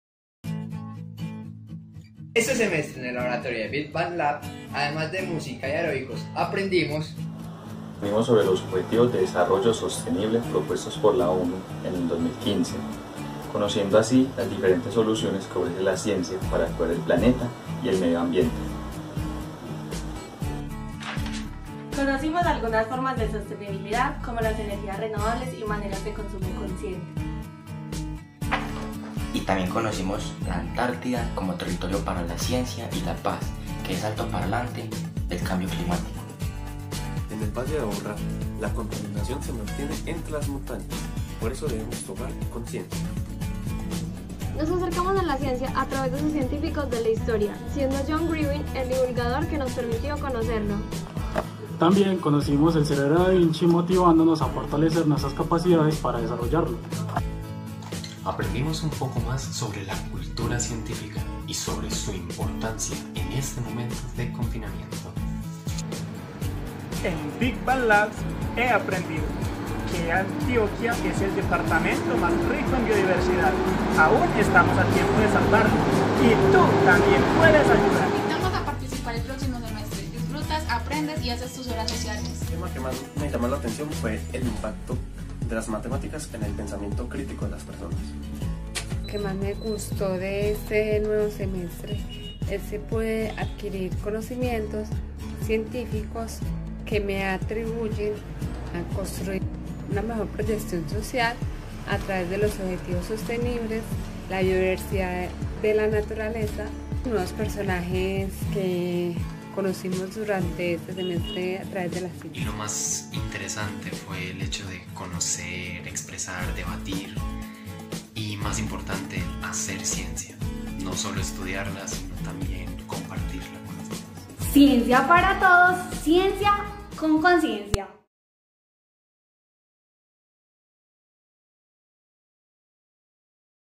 Decimo tercera muestra, Bigbang Lab
¿Te interesa la ciencia, la cultura, el medio ambiente y el mundo del cine?
Que estás esperando para ser parte de nuestros Laboratorios de Ideas. Por eso te invitamos para que te enteres de todo lo que hacemos durante el semestre. Te invitamos a ver la decimotercera muestra de los Labs de la Biblioteca.

Este es un vídeo realizado y producido por el Instituto Tecnológico Metropolitano ITM.